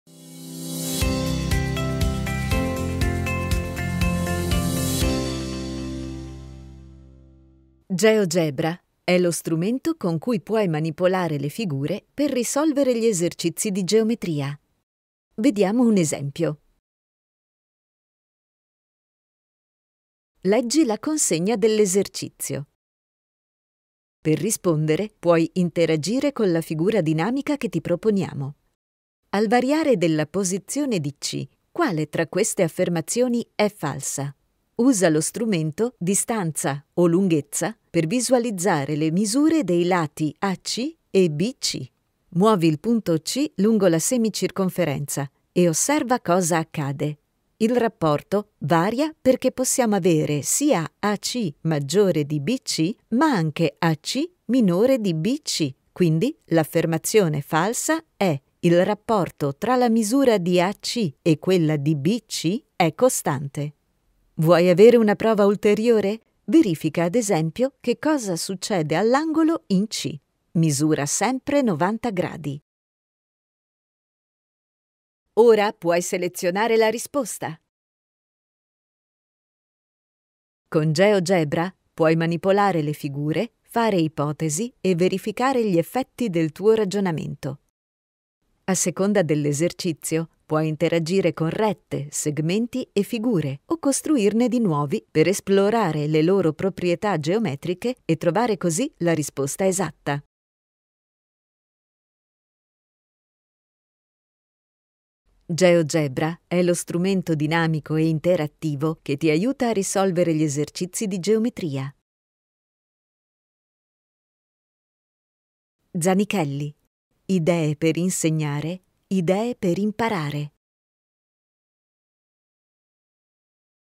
Tutor - Come si usa GeoGebra?
Video che spiega che cos'è GeoGebra, lo strumento per risolvere gli esercizi di geometria, e come è integrato nel Tutor di matematica.

Zanichelli editore S.p.a. 2021